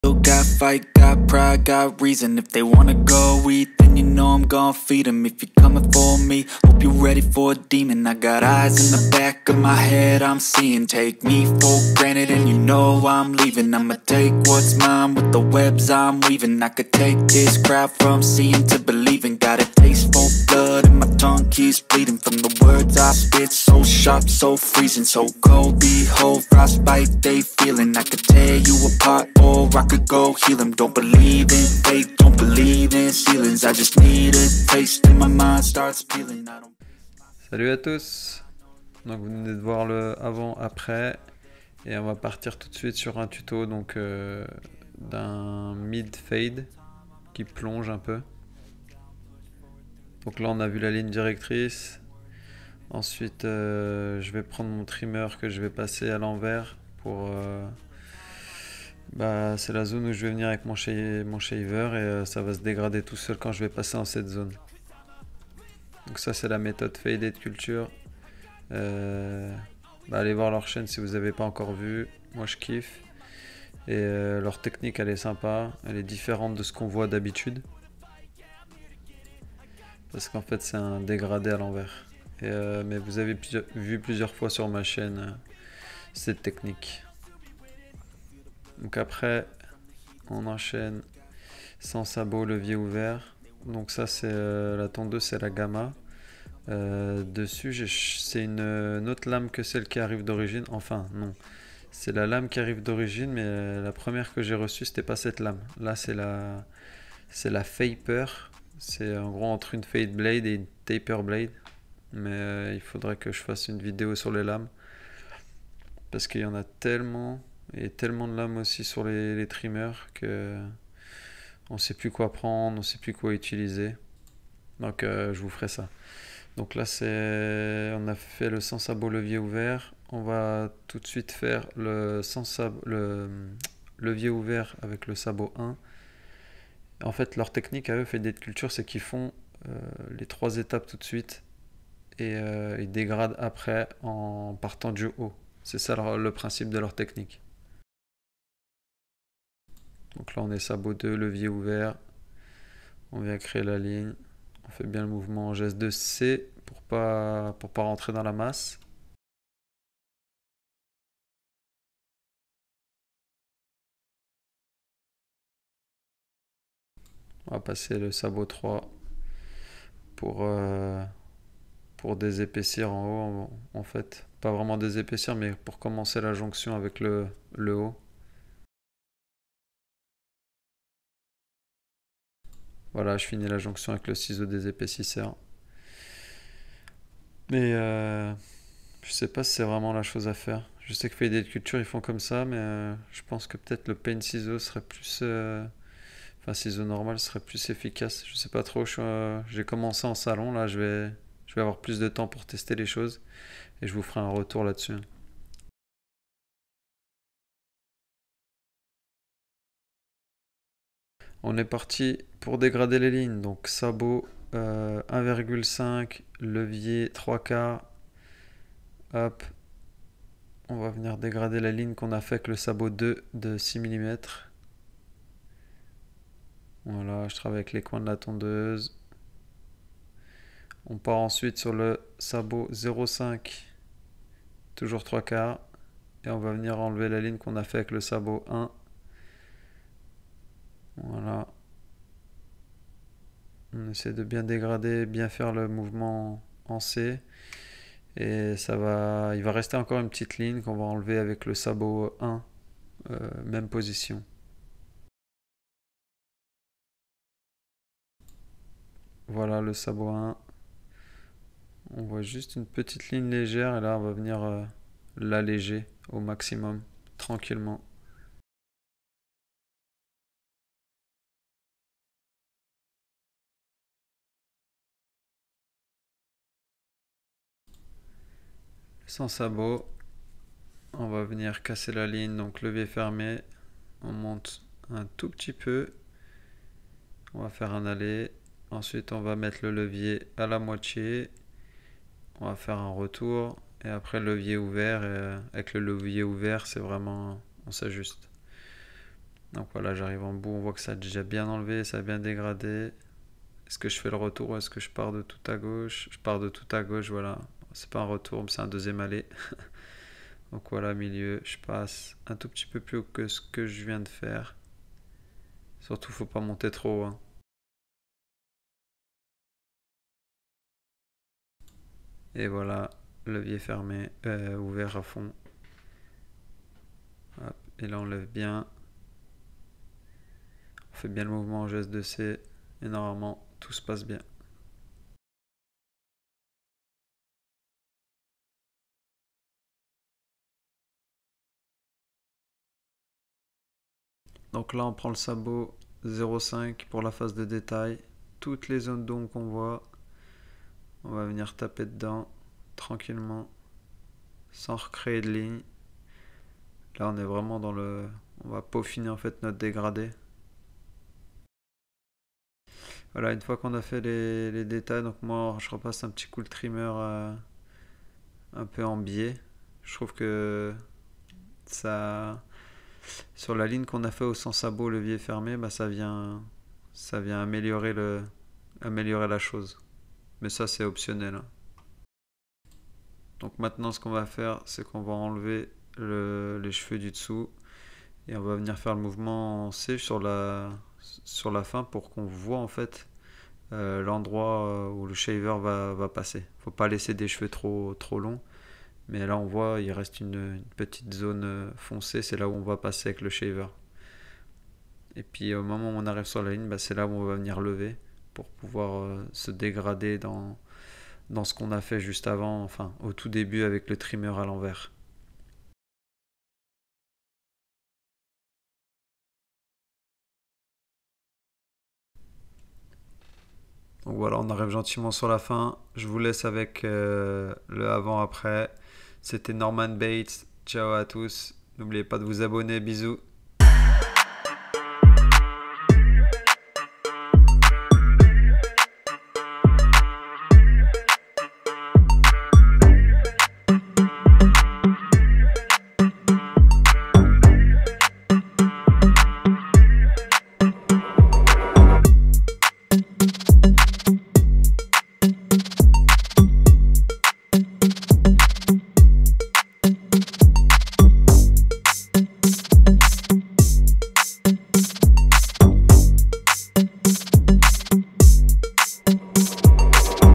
Transformation dégradé MIDFADE
Voici une transformation dégradé MIDFADE. J'ai utilisé la technique "faded culture".
Je te montre toutes les étapes pour réussir ce dégradé. J'utilise la tondeuse gamma boosted qui est pour moi la meilleur tondeuse du moment. J'utilise aussi le dernier trimmer de chez babyliss le lo pro fx, je fais encore quelques test pour vous expliquer les points fort et faible de cette tondeuse.

Matériels utilisés
Tondeuse : Gamma boosted
Tondeuse de finition : Lo pro FX lame skeleton gold
Tondeuse de finition 2 :
Rasoir électrique : Babyliss fx02
Sabot : wahl
Peigne : wahl speed comb blanc
Peigne rose : YS park 332
Lumière : Ring light Neewer
Gel de rasage : american crew
Brosse dégradé : Denman fade

Caméra :
Iphone 7